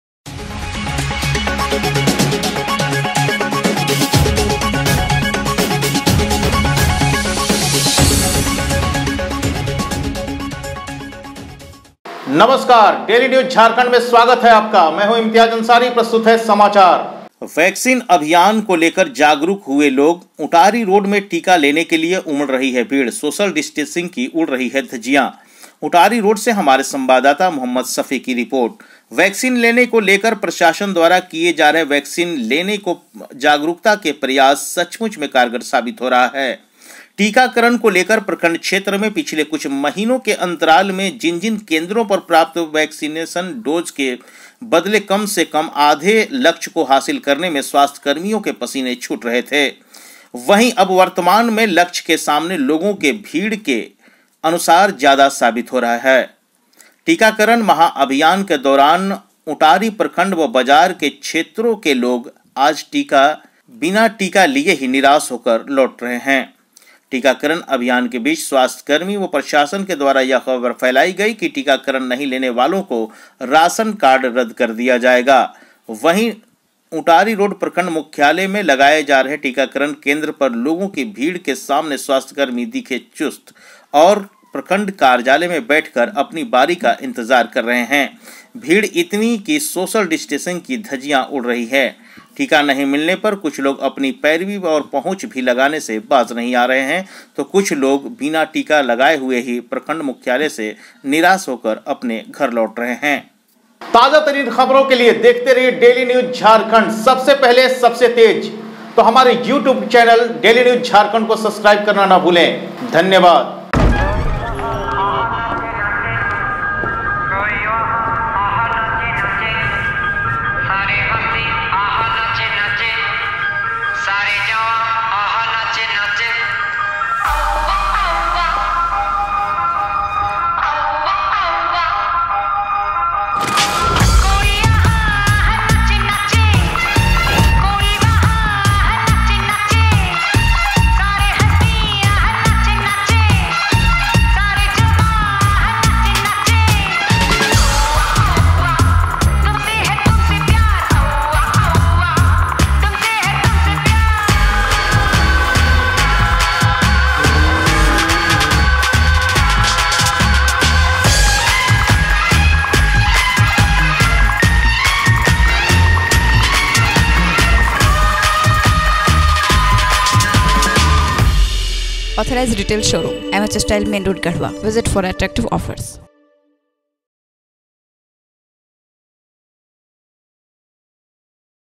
उंटारी रोड | टिका लेने के लिए उमड़ पड़ी भीड़, सोशल डिस्टेंस की उड़ी धज्जियां
वैक्सीन अभियान को लेकर जागरूक हुए लोग ,उंटारी रोड में टिका लेने के लिए उमड़ पड़ी भीड़, सोशल डिस्टेंस की उड़ी धज्जियां,

उंटारी रोड से मोहम्मद सफी की रिपोर्ट

वैक्सीन लेने को लेकर  प्रशाशन द्वरा किये जा रहे वैक्सीन लेने को जागरूकता के प्रयास सचमुच में कारगर साबित हो रहा है, टीकाकरण को लेकर प्रखंड क्षेत्र में पिछले कुछ महीने के अंतराल में जिन जिन केंद्रों पर प्राप्त वैक्सीन डोज के बदले कम से कम आधे लक्ष्य को हासिल करने में स्वास्थ्यकर्मियों के पसीने छूट रहे थे, वहीं अब वर्तमान में लक्ष्य के सामने लोगों के भीड़ के अनुसार ज्यादा साबित हो रहा है, टीकाकरण महाअभियान के दौरान उंटारी प्रखंड व  बाजार के क्षत्रो के लोग आज बिना टिका लिए निराश होकर लौट रहे है, टीकाकरण अभियान के बीच स्वास्थ्यकर्मी व प्रशाशन के द्वरा यह खबर फैलाई गई कि टीकाकरण नहीं लेने वालों को राशन कार्ड रद्द कर दिया जायेगा  वहीं अब उंटारी रोड प्रखंड मुख्यालय में लगाए जा रहे टीकाकरण केंद्र पर लोगों के भीड़ के सामने स्वस्थकरमी दिखे चुस्त,  लोग प्रखंड कार्यालय में बैठककर  अपने बारी का इंतजार कर रहे हैं, भीड़ इतनी के सोशल डिस्टेंस की धाजियां भी उड़ रही है, टिका नहीं मिलने पर कुछ लोग अपनी पैरवी व पहुँच भी लगाने से  बाज  नहीं आ रहे हैं तो कुछ लोग बिना टिका लिए हुए प्रखंड मुख्यालय से निराश हो वापस घर लौट रहे है,